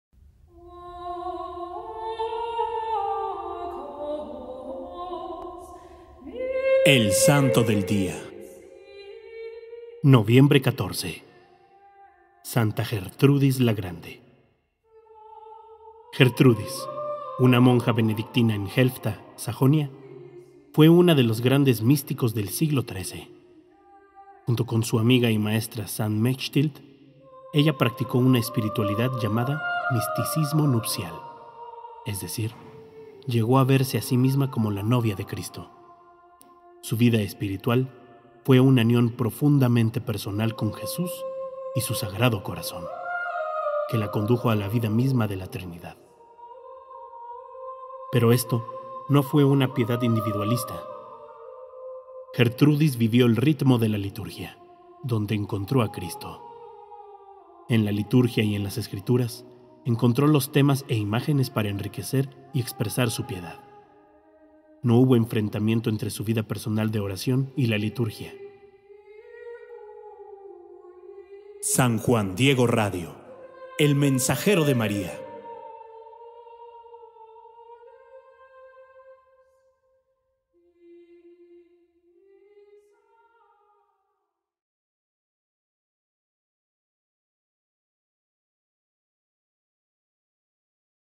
Santa Gertrudis la Grande
SANTO DE HOY, JUEVES NOVIEMBRE 14
Gertrudis, una monja benedictina en Helfta, Sajonia, fue uno de los grandes místicos del siglo XIII. Junto con su amiga y maestra Saint Mechtild, ella practicó una espiritualidad llamada "misticismo nupcial", es decir, llegó a verse a sí misma como la novia de Cristo. Su vida espiritual fue una unión profundamente personal con Jesús y su Sagrado Corazón, que la condujo a la vida misma de la Trinidad.

Pero esto no fue una piedad individualista. Gertrudis vivió el ritmo de la liturgia, donde encontró a Cristo. En la liturgia y en las Escrituras encontró los temas e imágenes para enriquecer y expresar su piedad. No hubo enfrentamiento entre su vida personal de oración y

la liturgia.
FUENTE:
TRADUCIDO POR GOOGLE TRANSLATOR